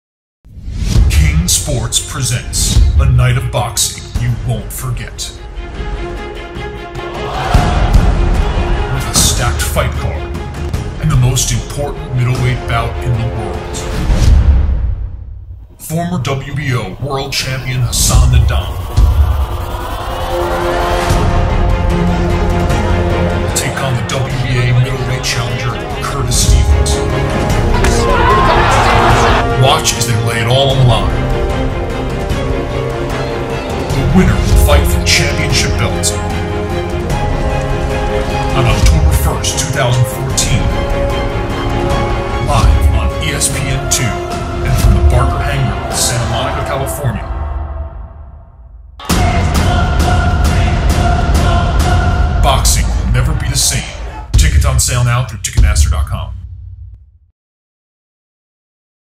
AN EPIC FIGHT NIGHT OFFICIAL PROMO
This is a promo video made exclusively with footage from our April 16, 2014 event. King Sports' next boxing event is on October 1, 2014, broadcast live on ESPN 2 from the Barker Hangar in Santa Monica / 9 PM EST - 6 PM PST.
The main event of the night is Hassan N'Dam 30-1 (18 KO) vs Curtis Stevens 27-4 (20 KO) for an IBF Eliminator fight (the winner fights for the world title).